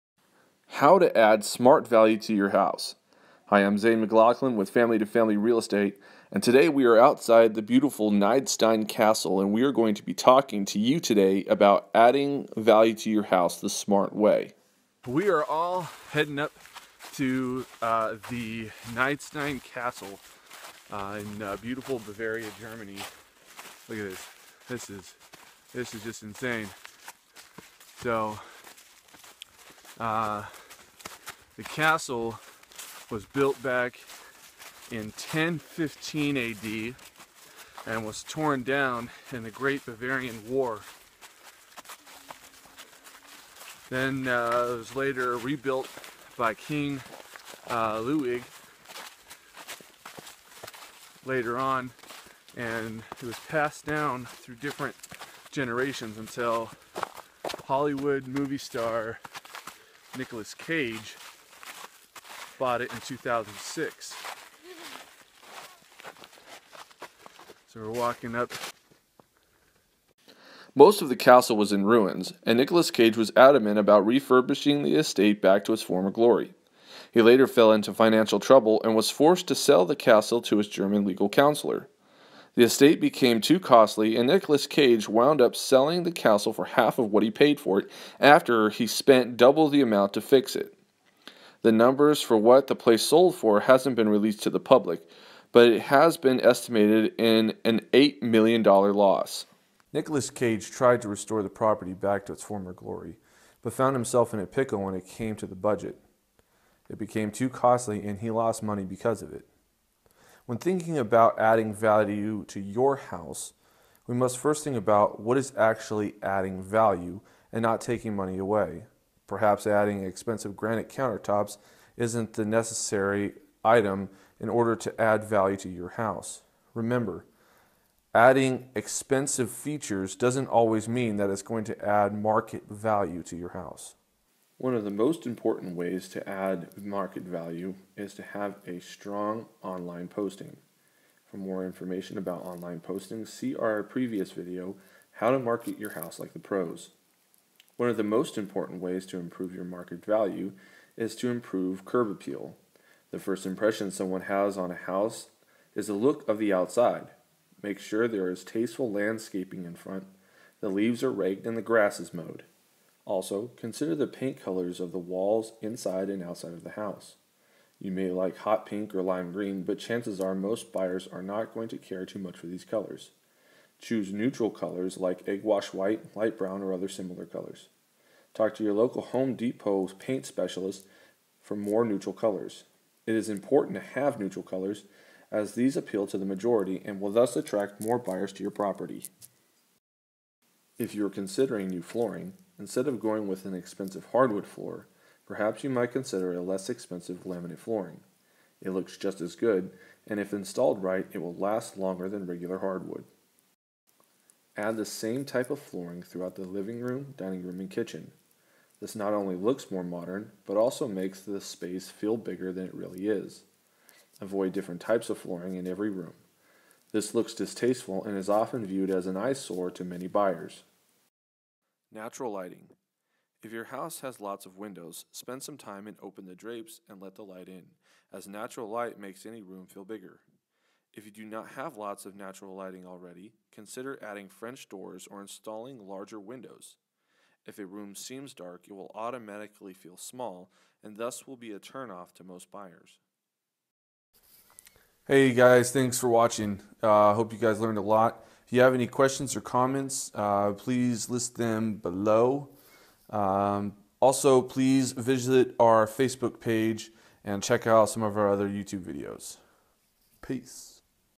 How to Add Smart Value to Your House
How to Add Smart Value to Your House. We visit the Neidstein castle, the one that Nicolas Cage bought back in 2006, but later had to sell it after he went over budget when he tried to have it renovated. We talk to you about how you can stay out of the same trouble, and how you can be smart about adding value to Your House before you sell it.